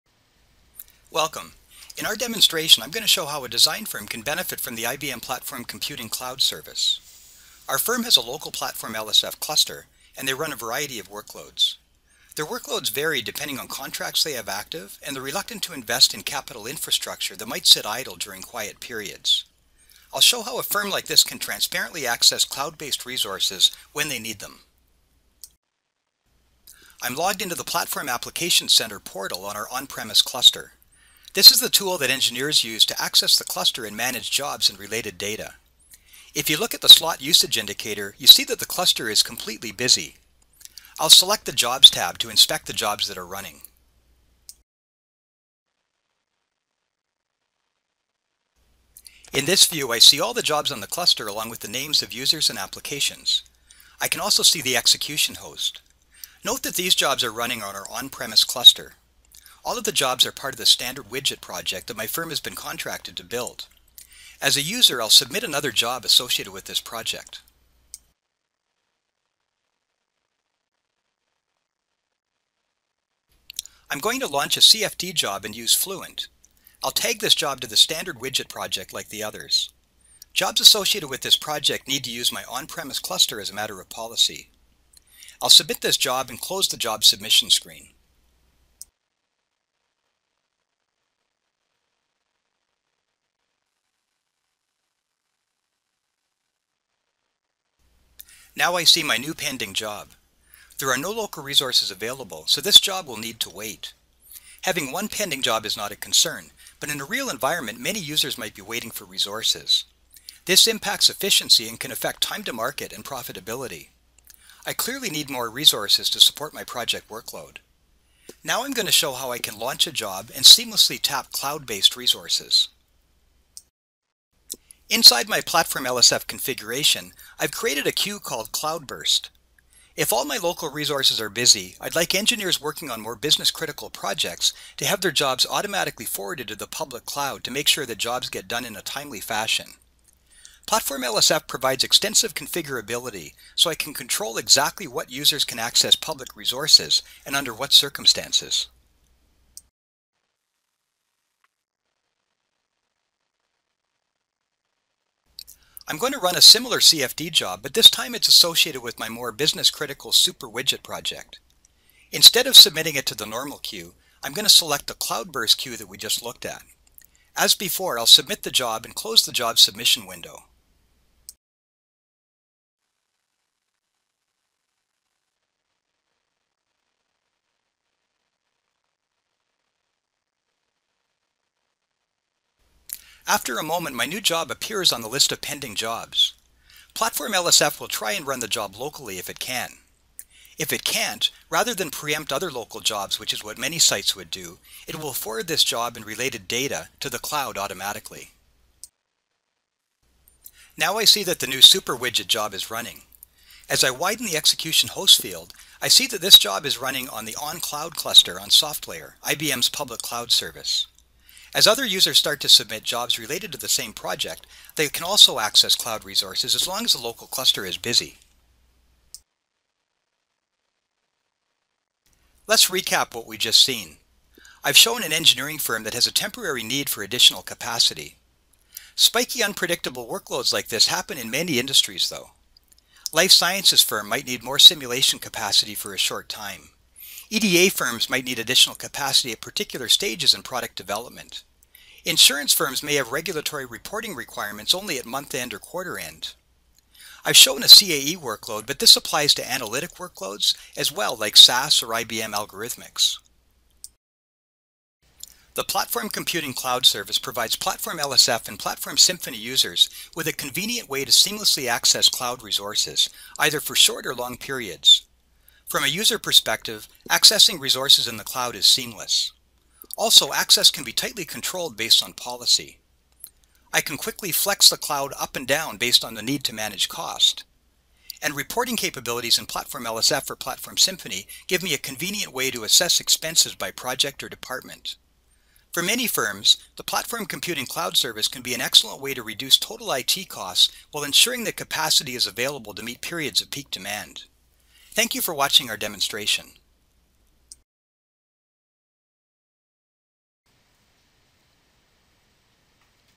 IBM Platform Computing Cloud Service Demonstration
This demonstration shows how an organization using IBM Platform Computing workload managers can easily tap capacity in the IBM SoftLayer cloud to more cost effectively handle variable or periodic workloads.